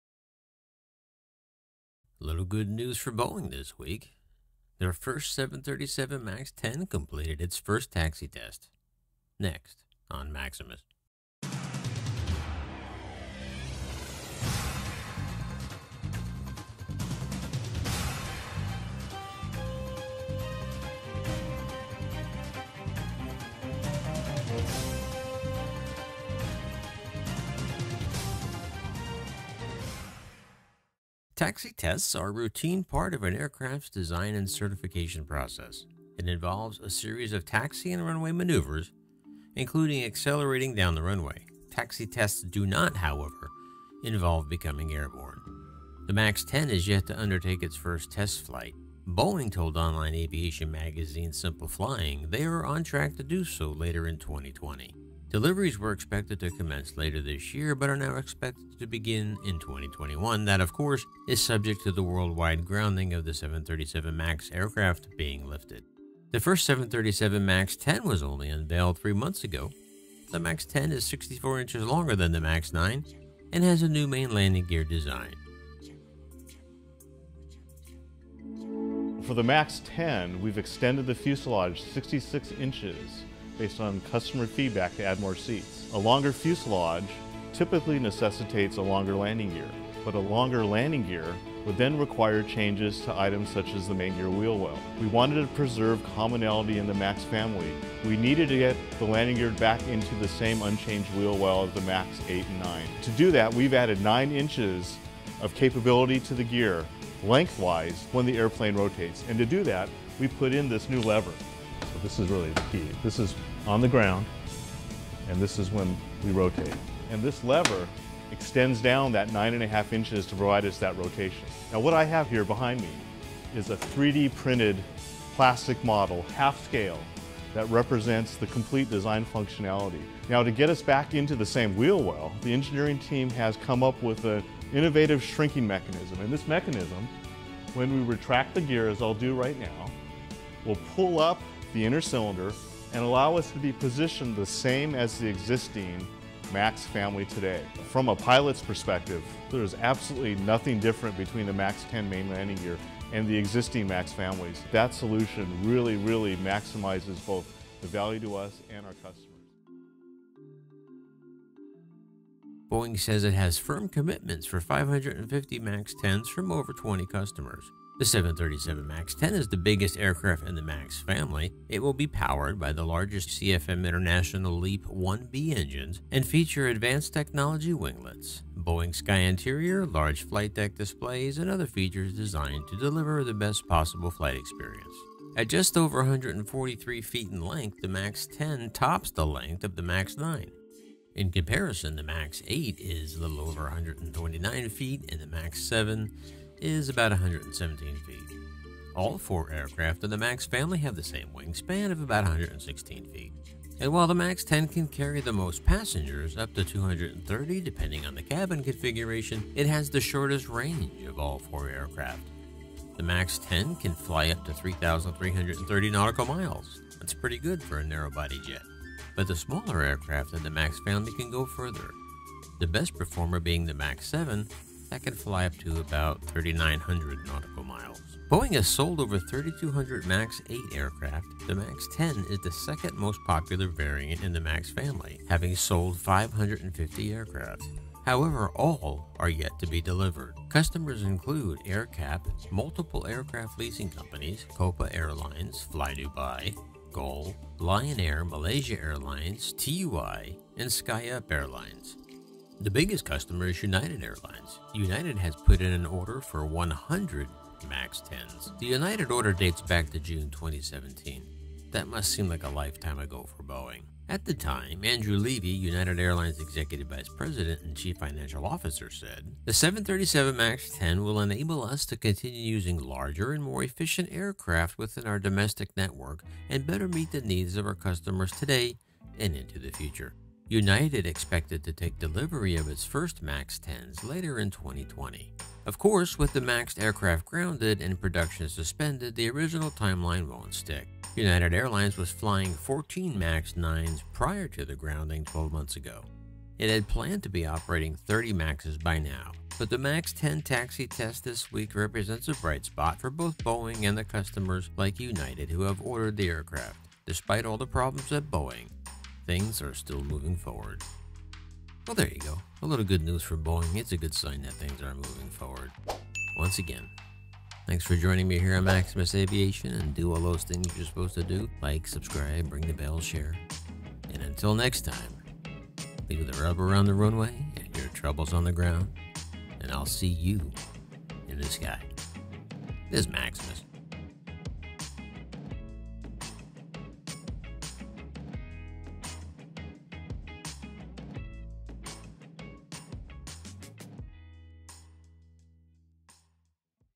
Boeing Max 10 Takes to the Runway for the First time for its official Taxi Test.
Amid continuing bad news, Boeing finally has a bit of hope.  The Max 10 its beggest of the Max family too its first test ride.  Albeit not in the air it was put through its paces for the Taxi test.